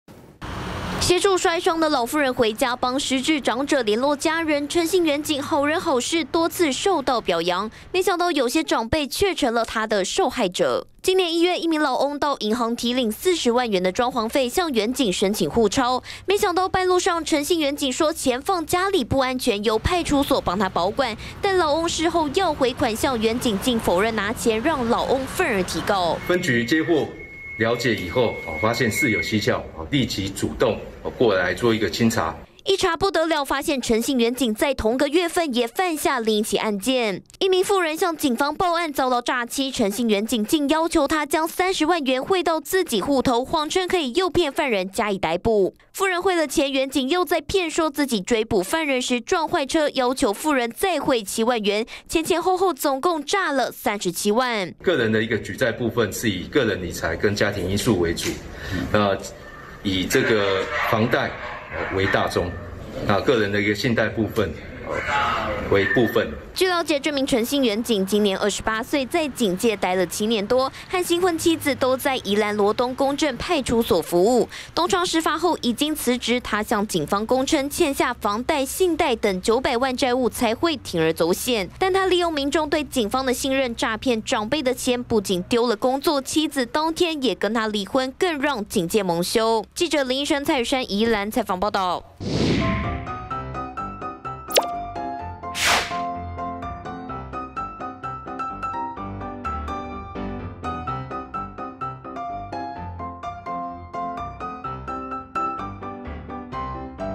【整點精華】20210207 男警欠債生歹念 連騙2長者77萬元
宜蘭羅東一名陳姓警員，因為欠下大筆債務，涉嫌在執行春節護鈔專案時，心生歹念，誆騙老翁40萬元；而後承辦詐騙案件時，誆騙被害婦人37萬元。東窗事發後，他已經離職，也和任職同派出所的女警妻子離婚，全案依貪污罪函送法辦。